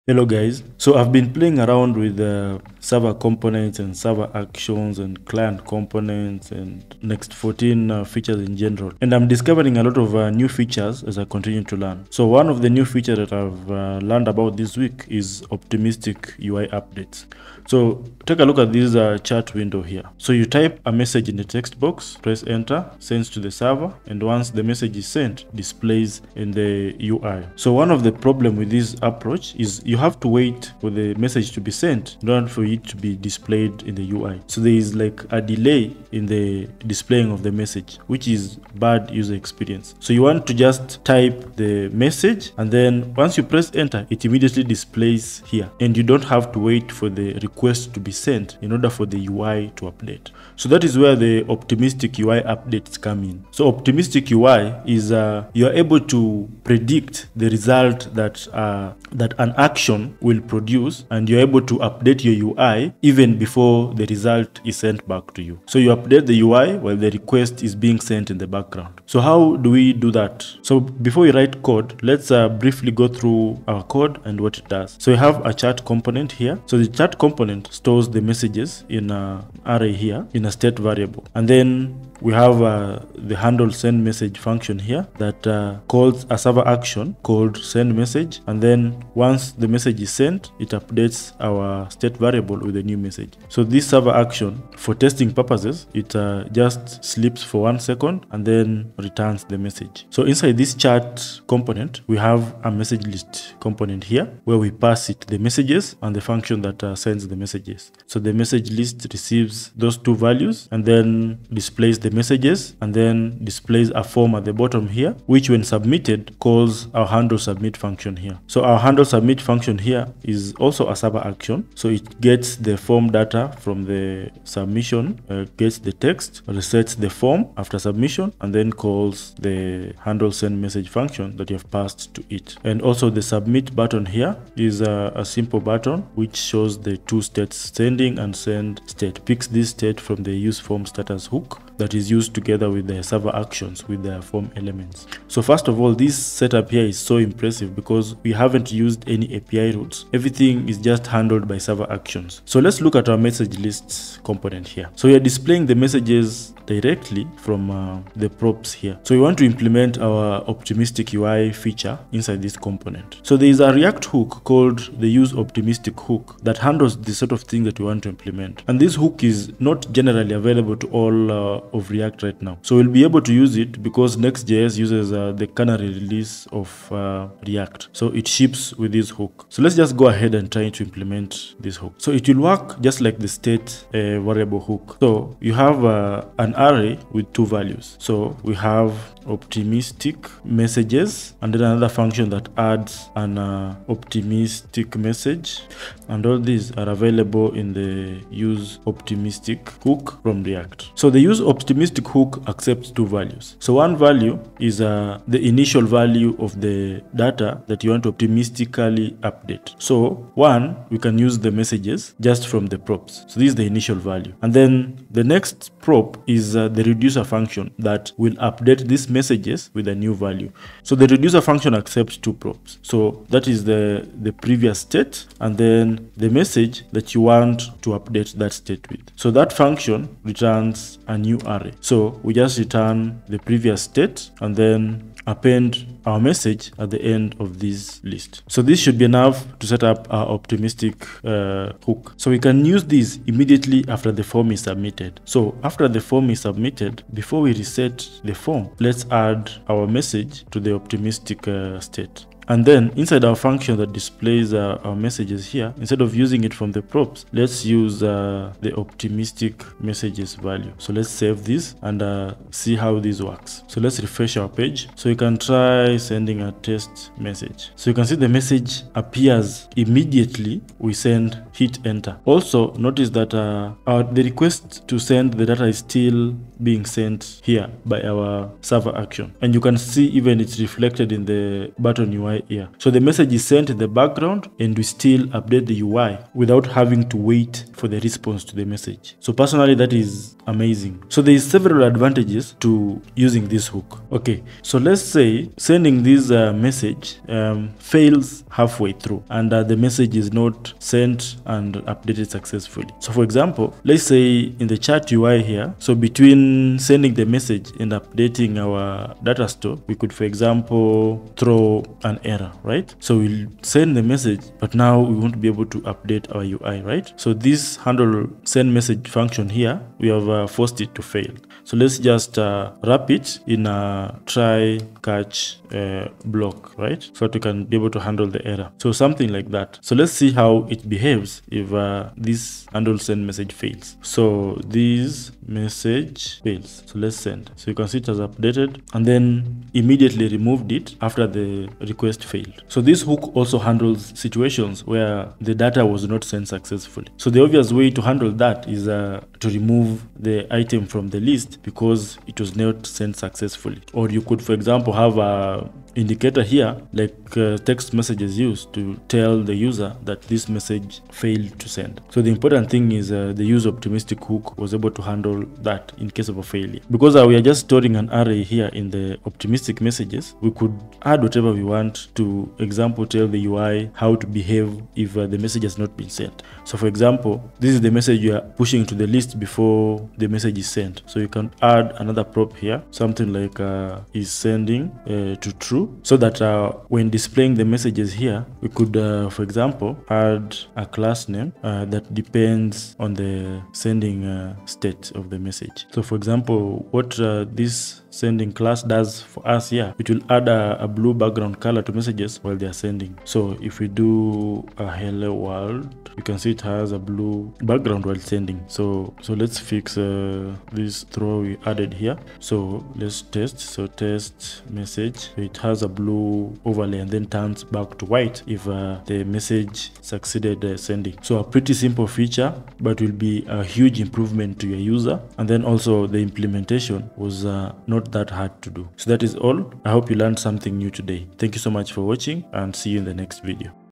Get rid of loading states using this react hook.
The useOptimistic hook comes in handy when you want to update your UI immediately while sending requests in the background.